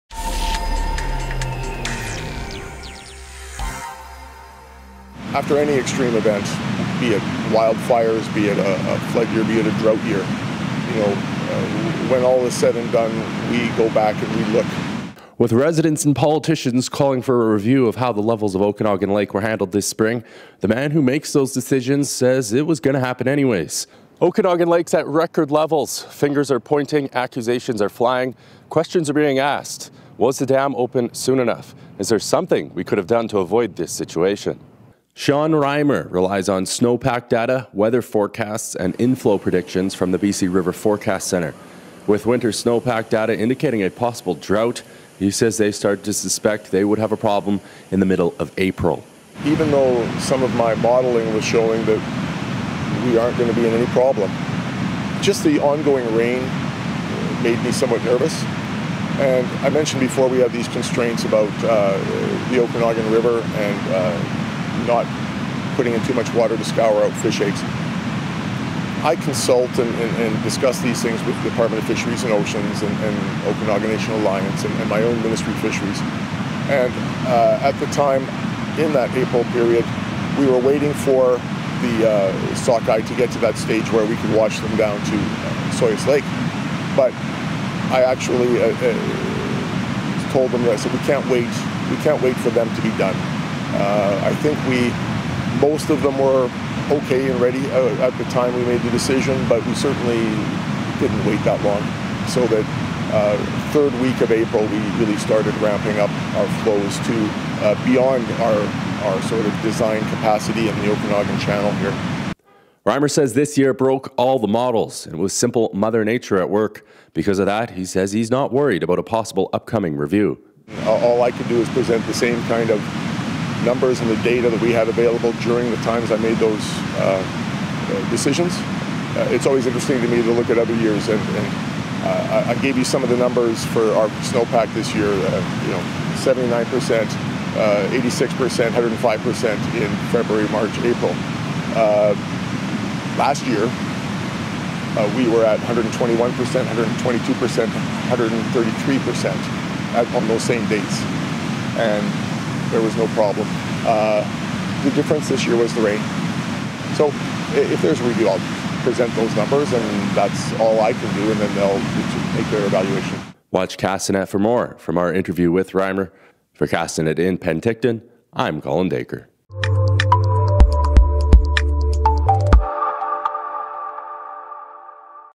This year's Okanagan spring melt will be studied
The man in charge of the flow of water out of Okanagan Lake says they will be looking back at the data to try to improve, but maintains this years episode of flooding was unavoidable